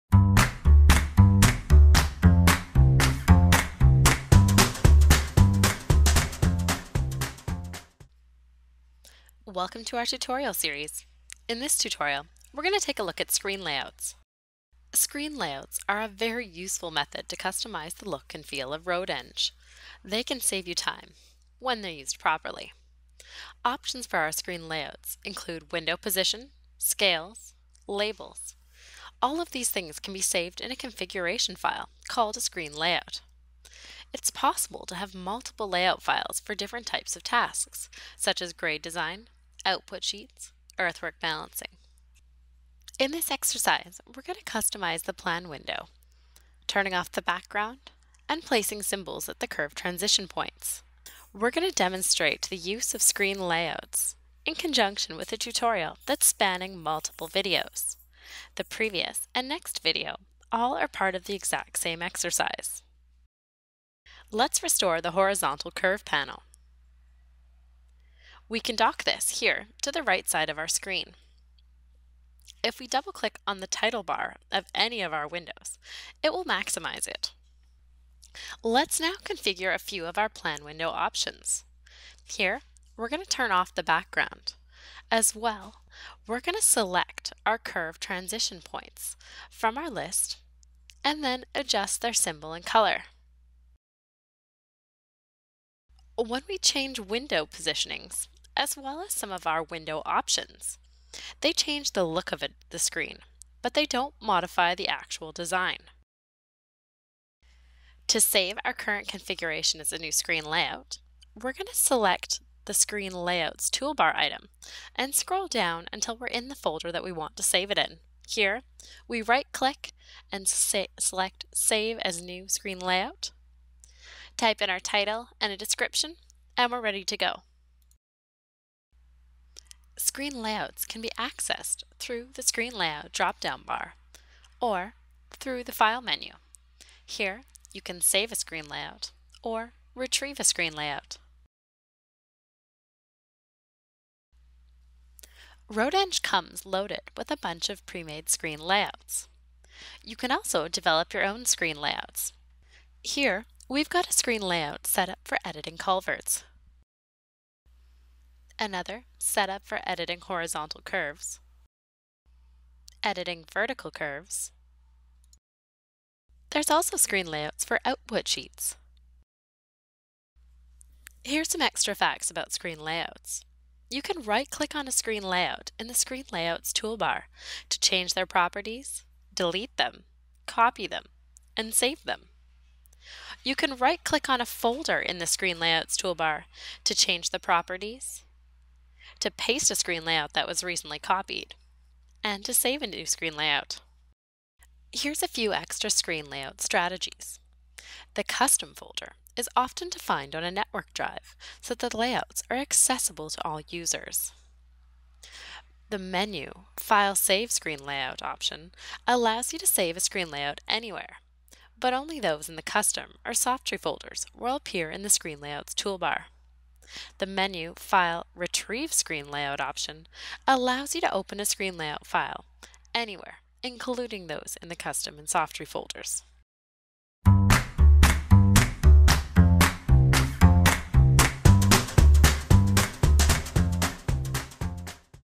Screen Layouts
This exercise will demonstrate the ability to create and save custom screen configurations with RoadEng and Terrain Tools.